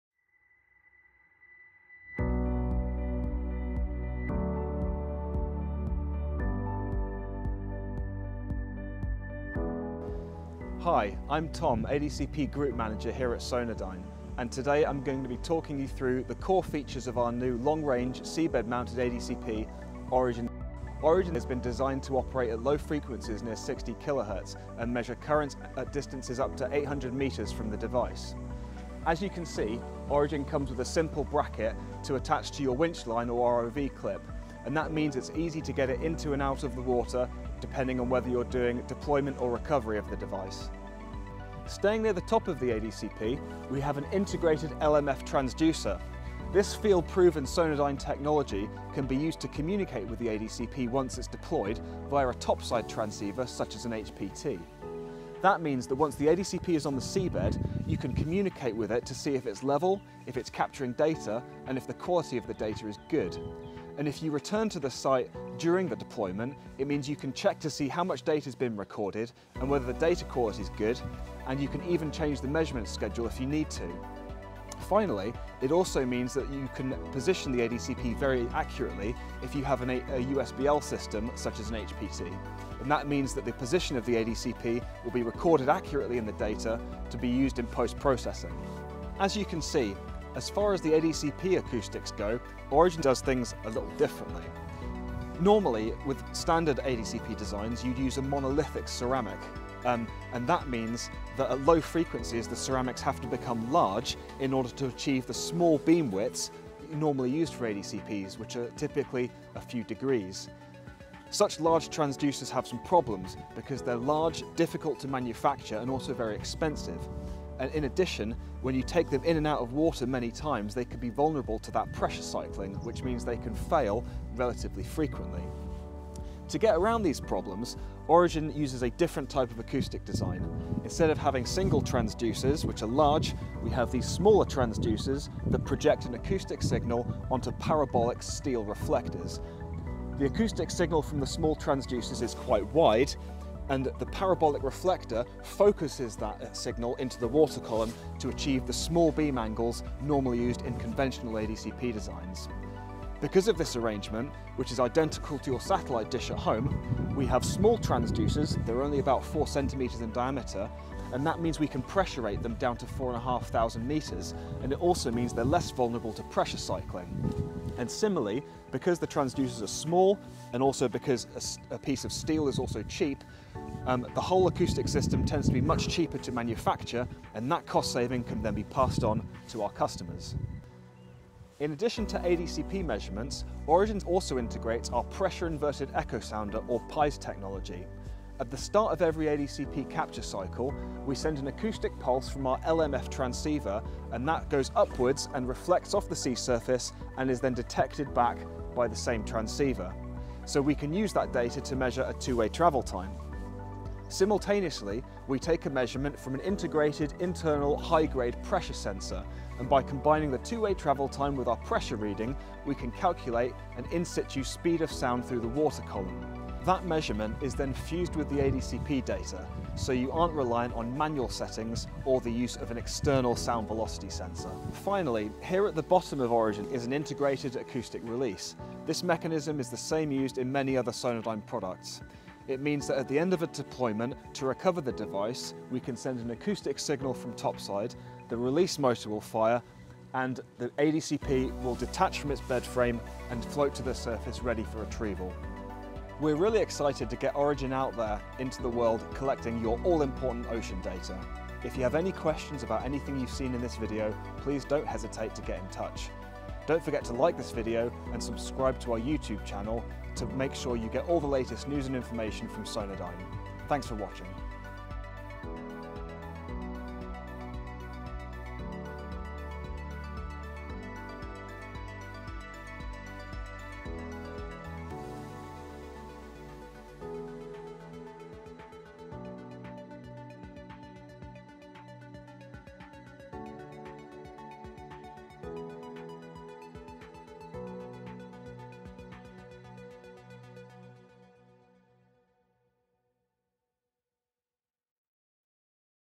Origin 65 - Long range ADCP
Origin 65 boasts a unique acoustic design that increases robustness, reduces cost, all while maintaining outstanding current profiling performance. The integrated modem allows for remote actions, whilst the PIES functionality delivers high-precision time-of-flight and average in-situ sound velocity data. Origin 65 is compatible with our Edge computing environment, allowing users to upload processing apps to Origin 65 that optimise the data for their specific application.

Timestamps
00:10 - Introduction
00:16 - Reason 1
00:42 - Integrated comms
01:30 - Unique acoustic design
03:04 - Integrated PIES
03:45 - Integrated release
04:12 - Get in touch

Useful Links